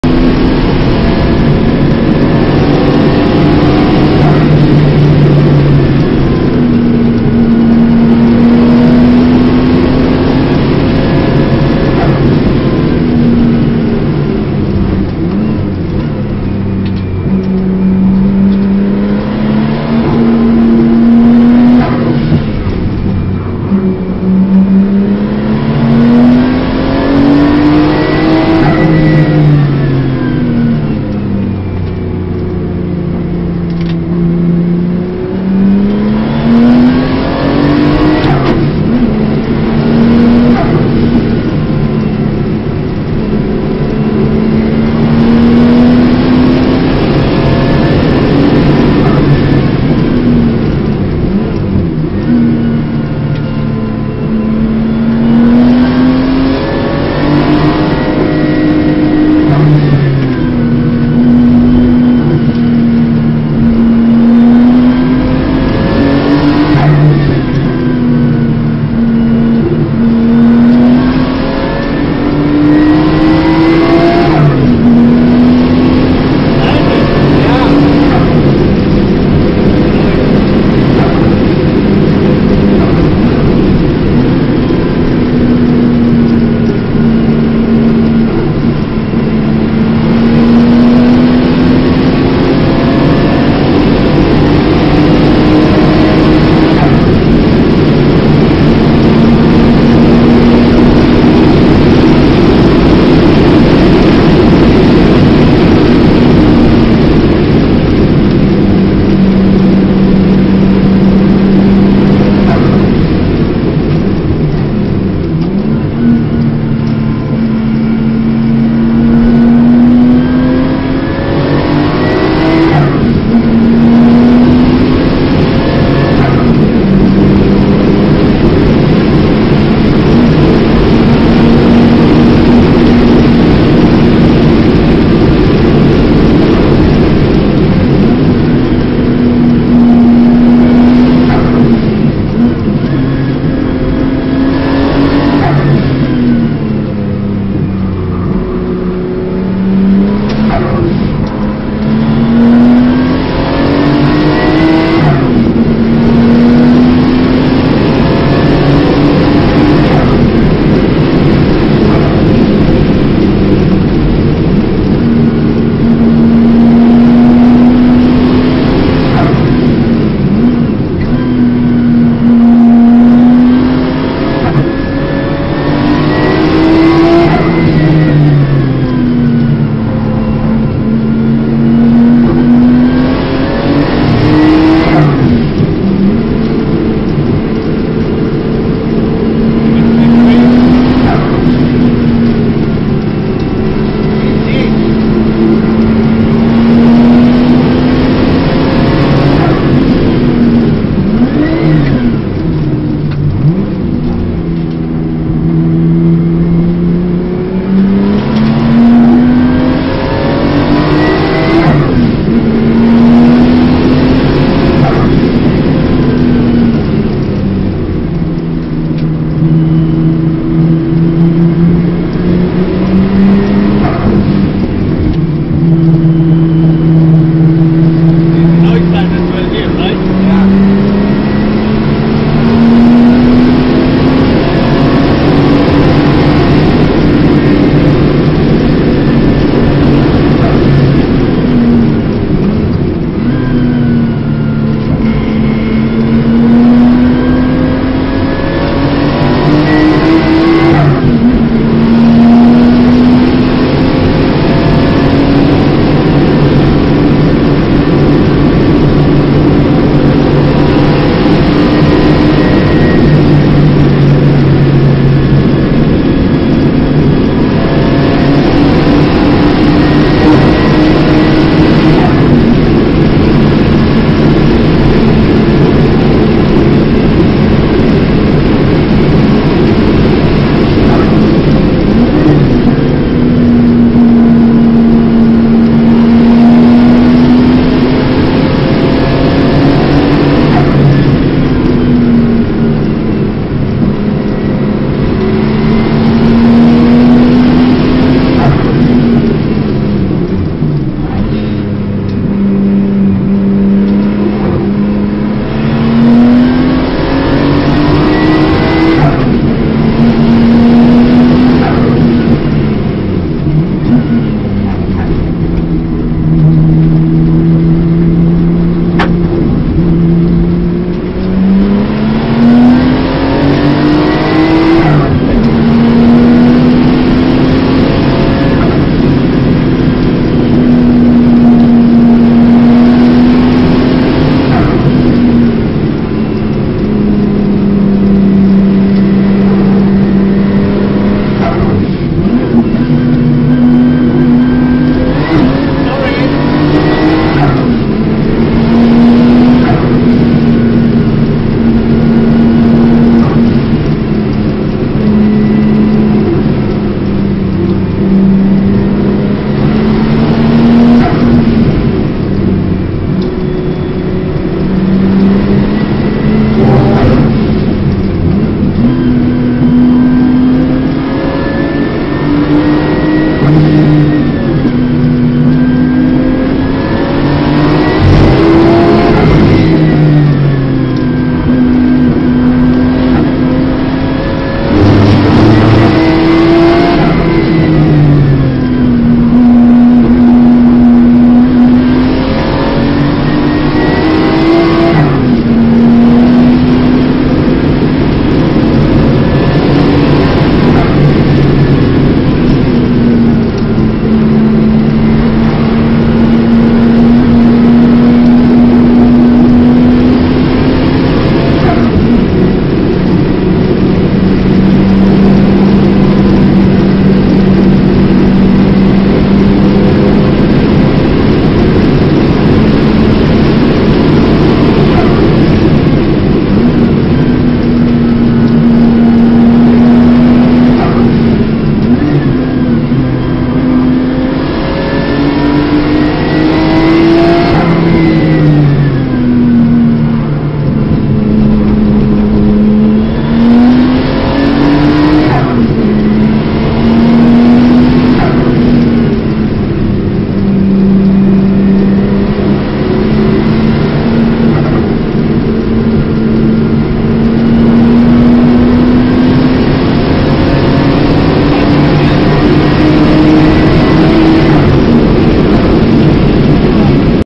PORSCHE 930 TURBO RWB onboard Nurburgring Lap
One of my best experience on the Nordschleife!
This was my second lap with this 930 turbo RWB with a few wet sections on the ring and I believe this car can go muck quicker!

The sound and the feeling it’s absolutely sensational and I hope you enjoyed watching the Lap ;)